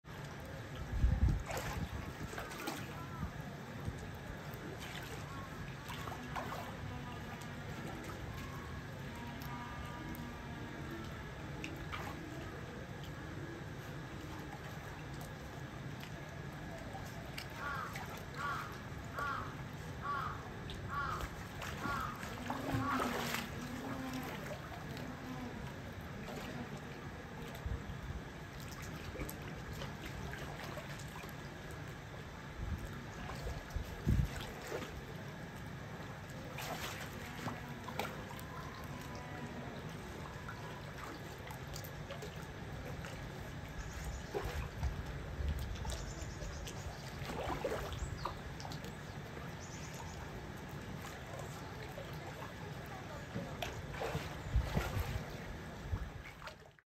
動く影が気になる ジェンツーペンギン
ジェンツーペンギン　ほい♂

水中でアクリル越しの事が多い　ほいですが、この時は　動く影をパクパクしていました。